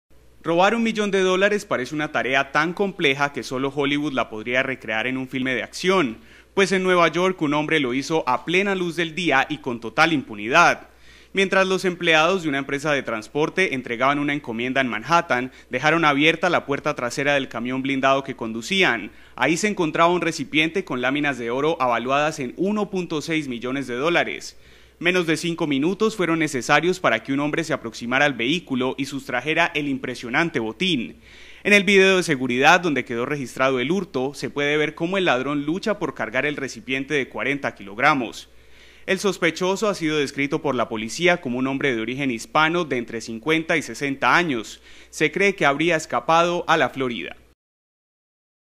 Increíble, hombre roba 1 6 millones de dólares en oro a plena luz del día
Robar un millón de dólares parece una tarea tan compleja que solo Hollywood la podría recrear en un filme de acción, pues en Nueva York un hombre lo hizo a plena luz del día y con total impunidad.